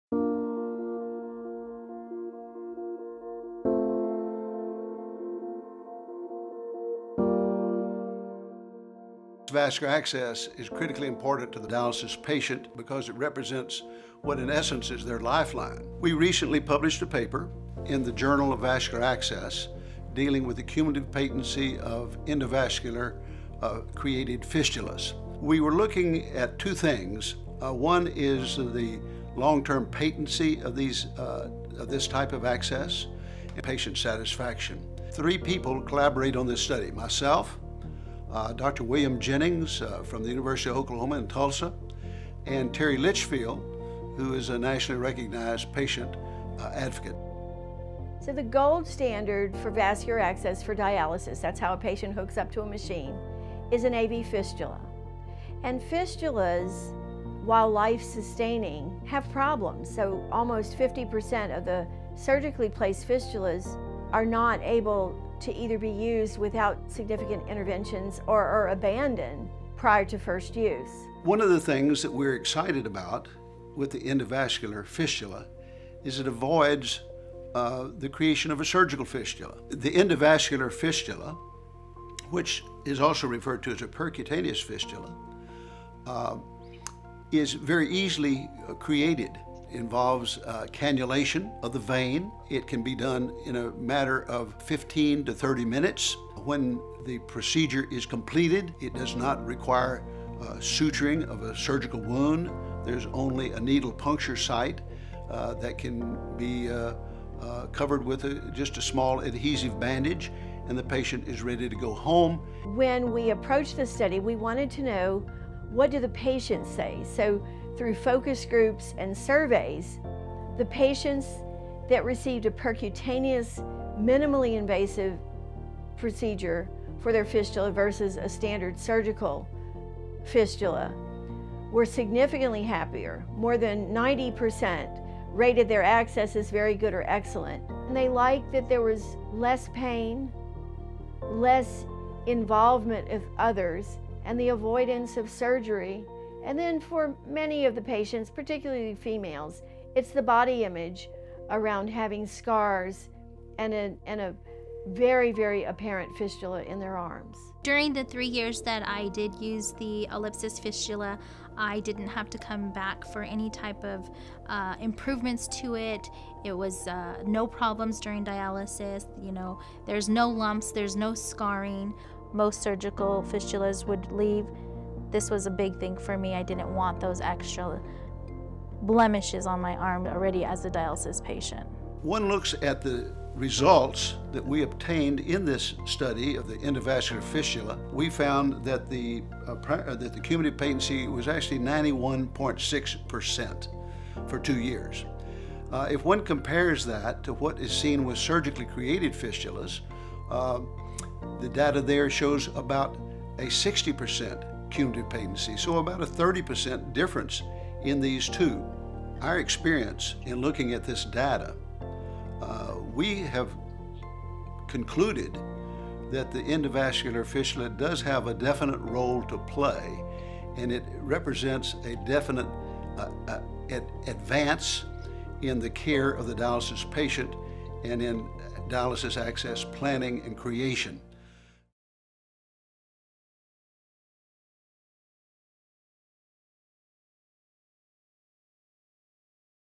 First Two-Year Published Data on an Endovascular AV Fistula for Dialysis Access
A study published in the Journal of Vascular Access shows high levels of patient satisfaction with the Ellipsys Vascular Access System, a minimally invasive approach to creating vascular access for kidney patients who require hemodialysis. In addition, researchers found that nearly 92% of fistulas created with Ellipsys were still functional after two years. This landmark study is the first published two-year data for any endovascular AV fistula procedure.